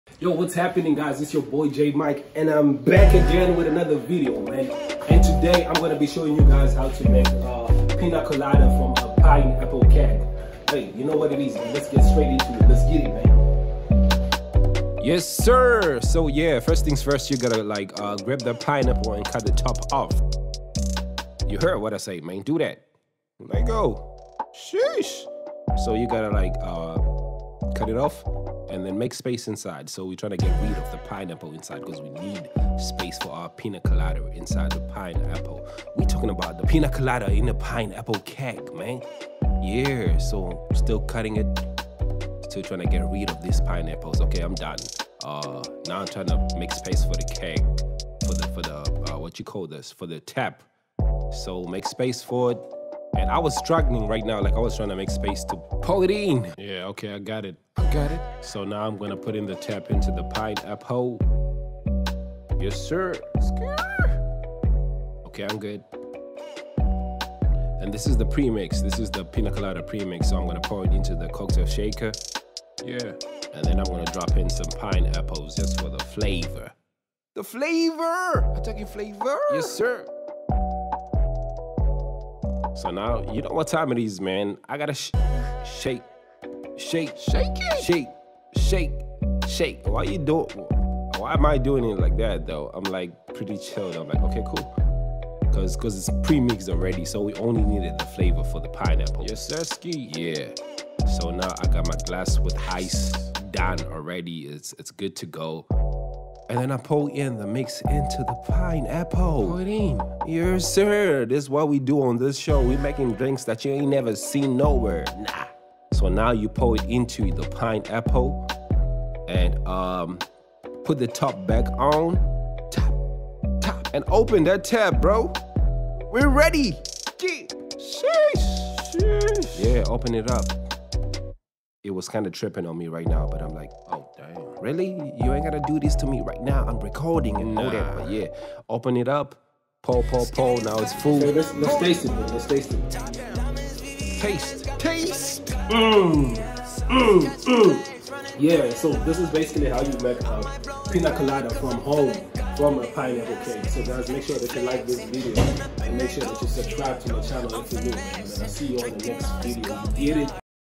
Pina Colada in Pineapple Keg at Home
How to easily make a Pineapple Keg at home.